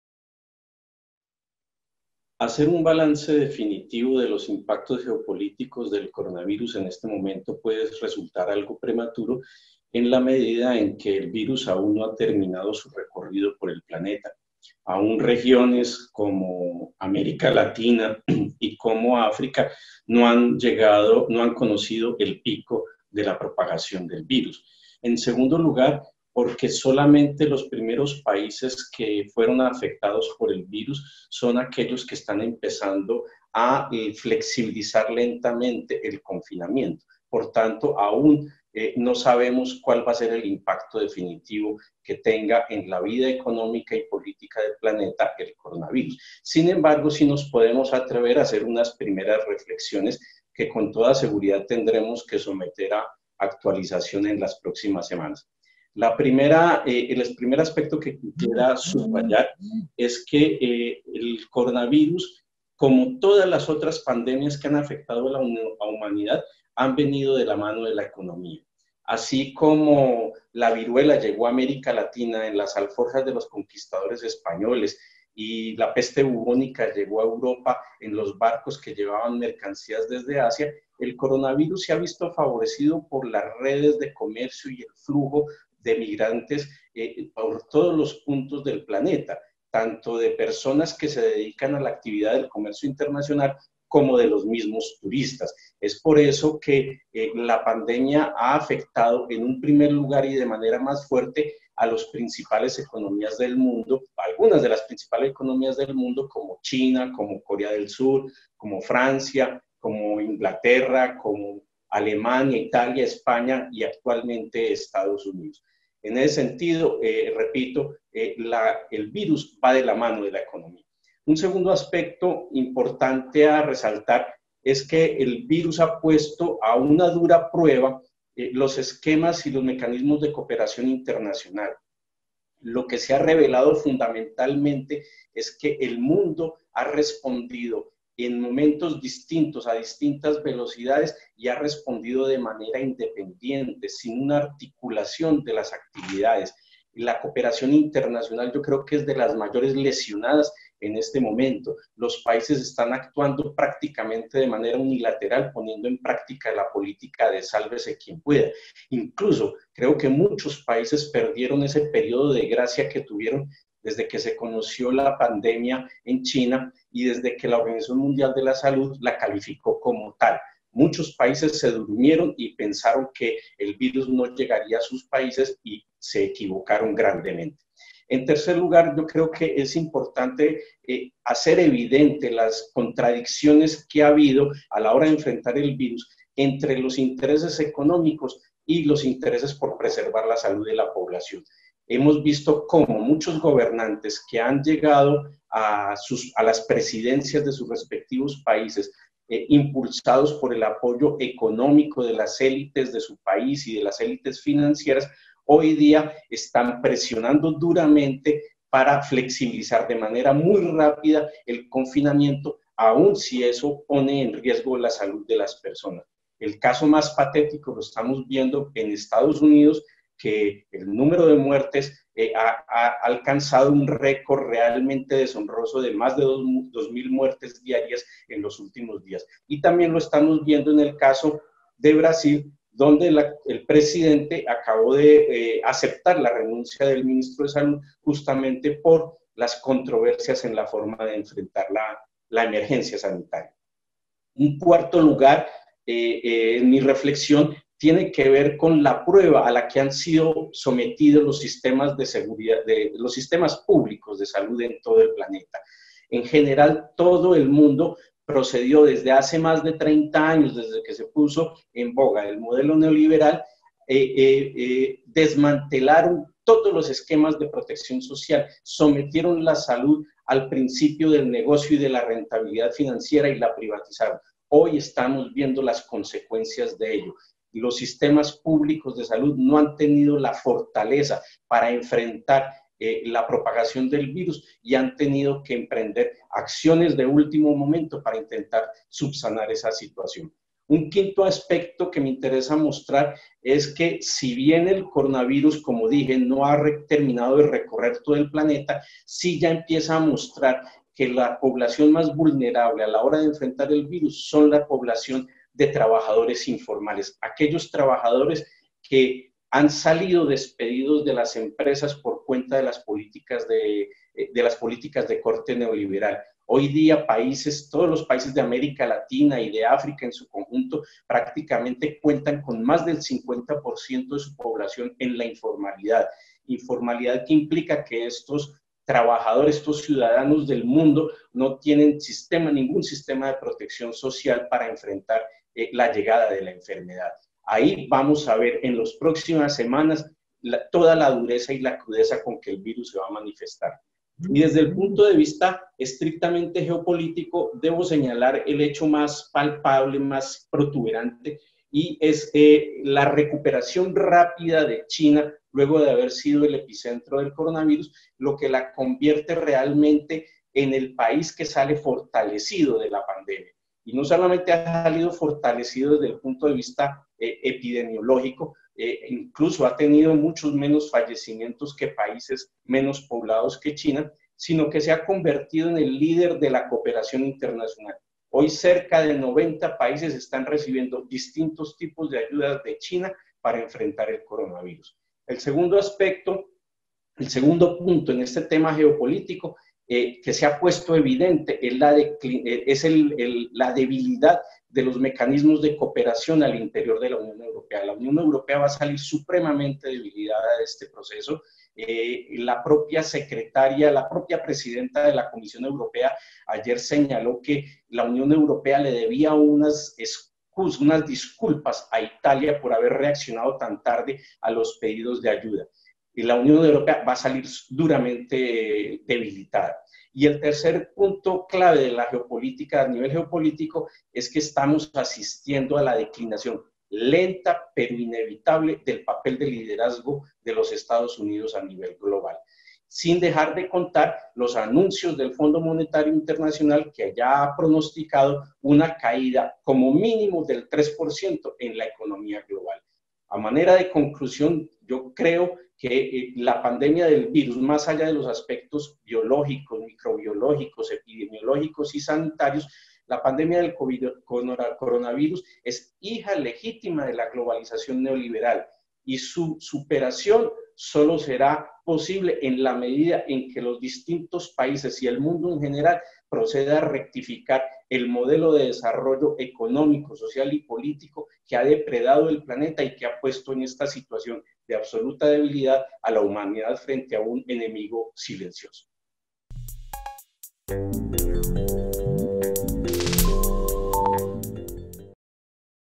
Reflexión geopolítica en la actual crisis del Coronavirus; globalización y política social.
En la Videocolumna de la semana, el profesor de la UdeADerechoyPolítica, Javier Sánchez, propone diferentes reflexiones en materia de geopolítica 🗺 que abordan la crisis del Coronavirus. El profesor invita a pensar temas como cooperación internacional, economía, salud y trabajo en medio de la pandemia.